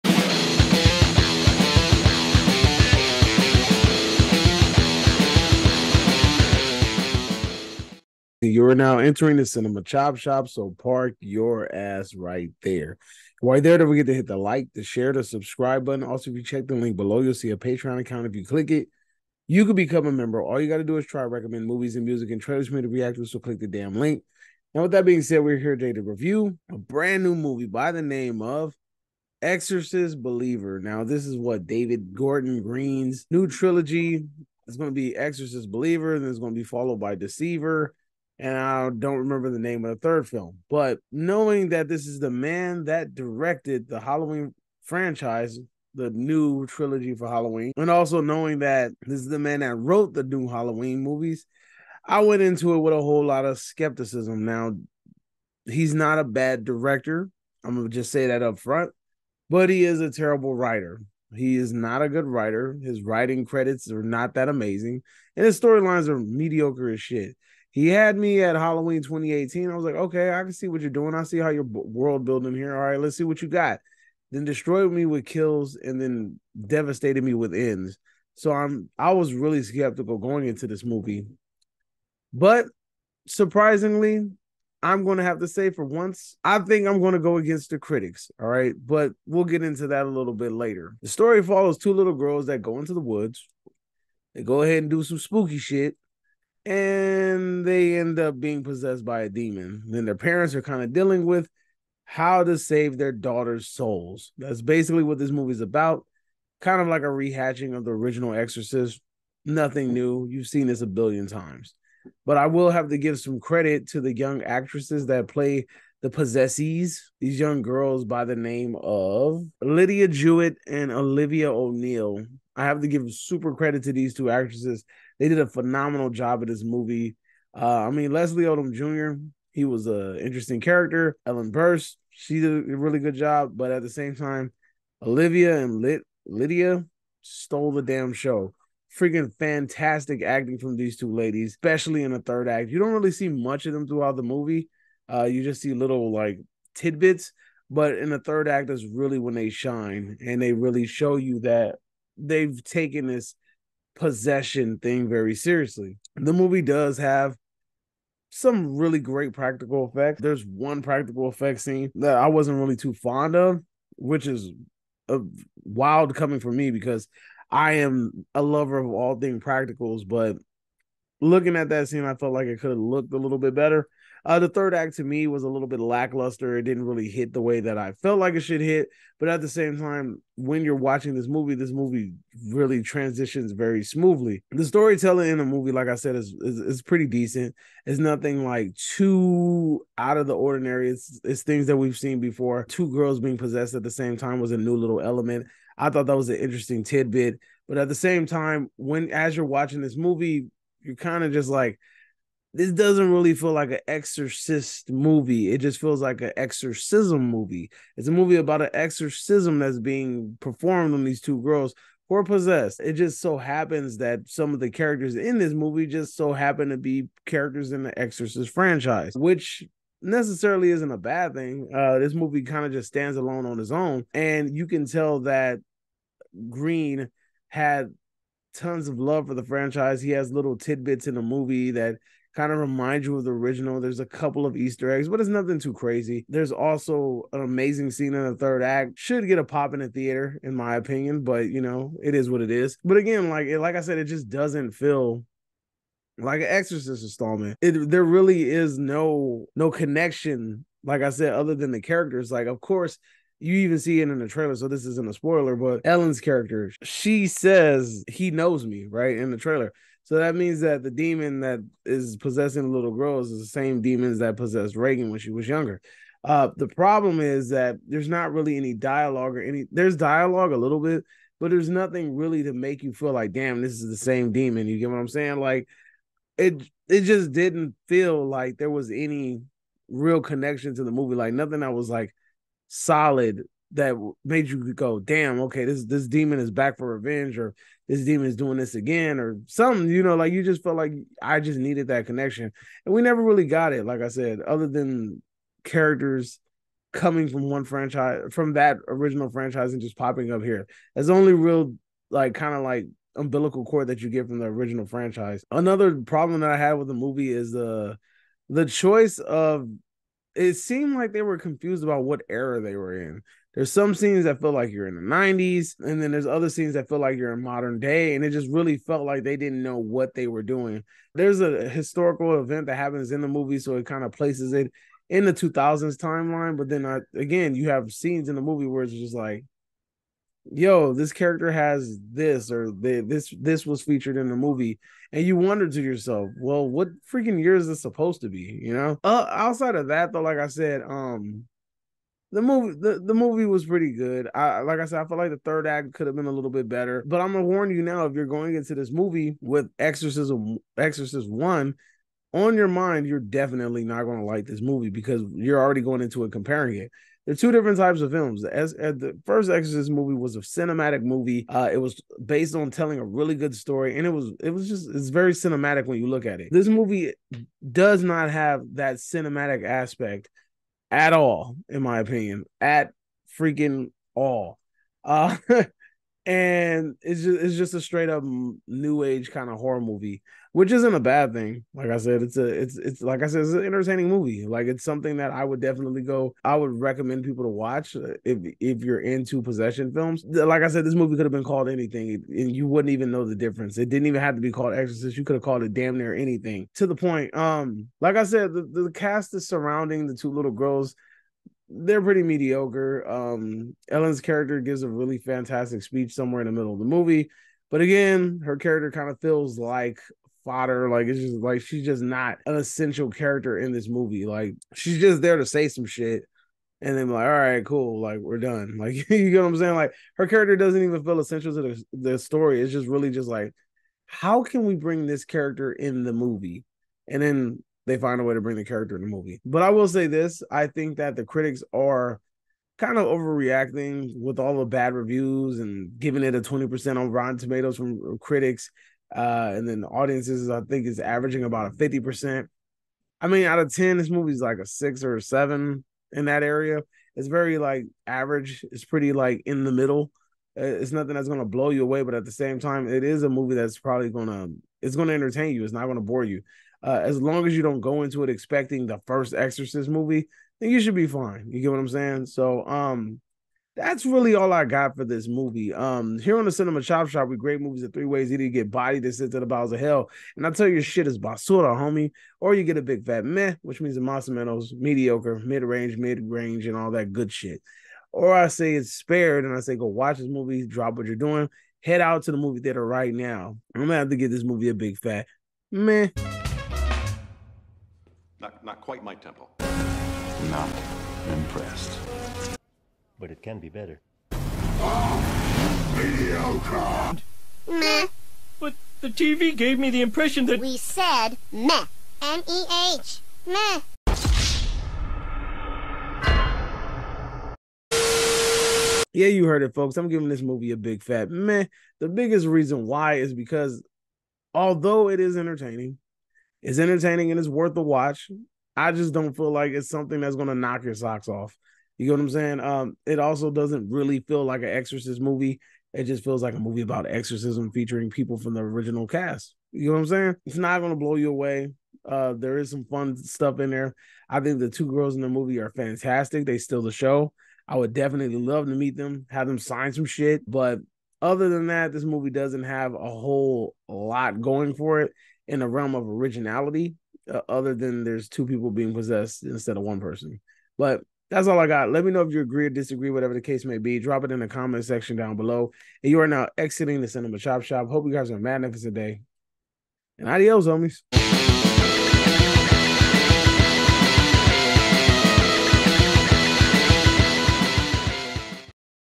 THE EXORCIST: BELIEVER x REVIEW || ARE THE CRITICS EXAGGERATING ?
Today I review the first installment to the new Exorcist trilogy directed by David Gordon Green.